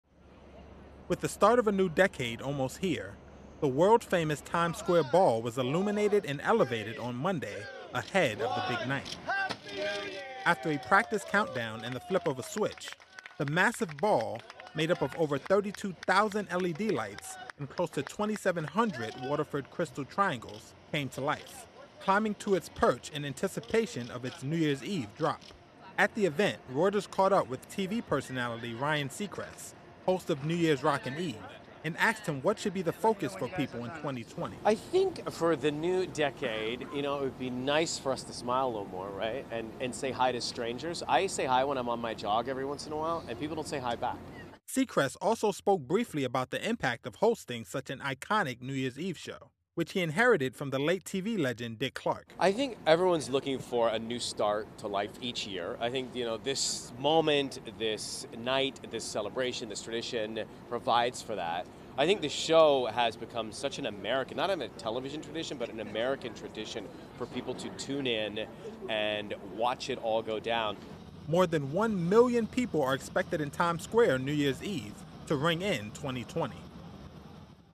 Times Square gears up for its annual NYE festivities
Ahead of the annual New Year's Eve ball drop in Times Square, a final test is completed and Reuters talks to TV personality Ryan Seacrest before the big night. Freddie Joyner has more.